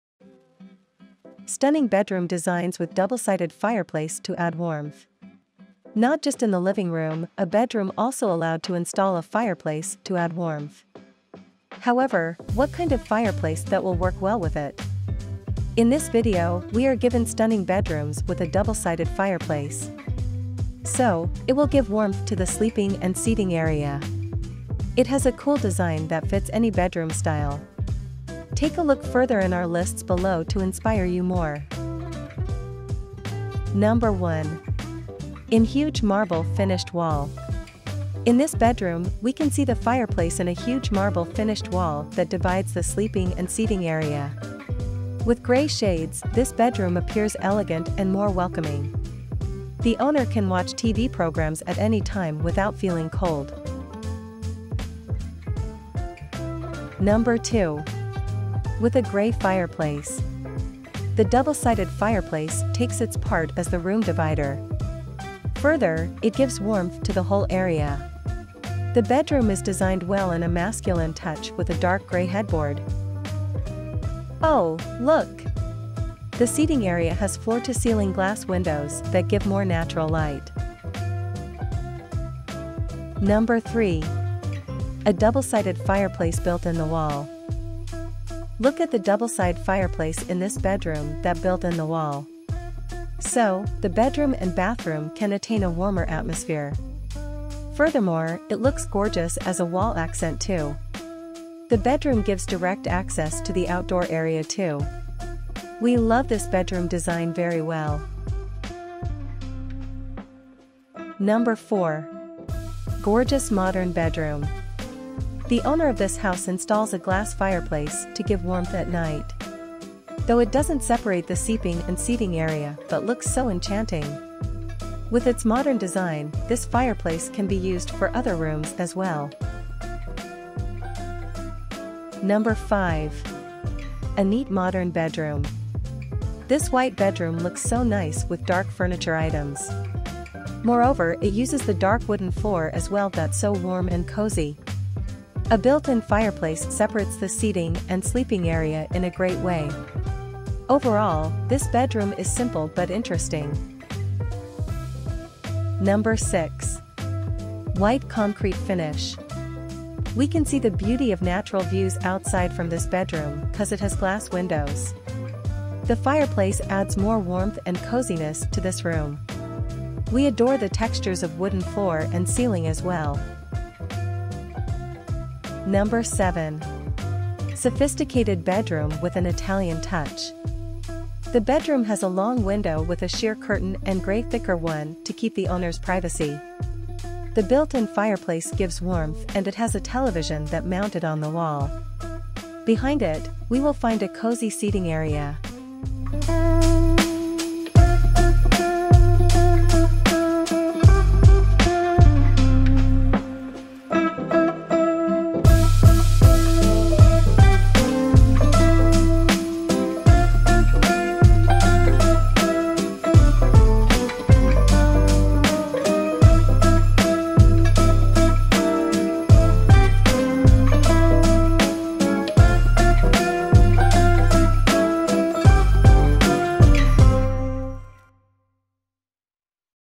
Stunning Bedroom Designs Ideas with Double-sided Fireplace
Not just in the living room, a bedroom also allowed to install a fireplace to add warmth. However, what kind of fireplace that will work well with it? In this video, we are given stunning bedrooms with a double-sided fireplace.

So, it will give warmth to the sleeping and seating area. It has a cool design that fits any bedroom style. Take a look further in our lists below to inspire you more!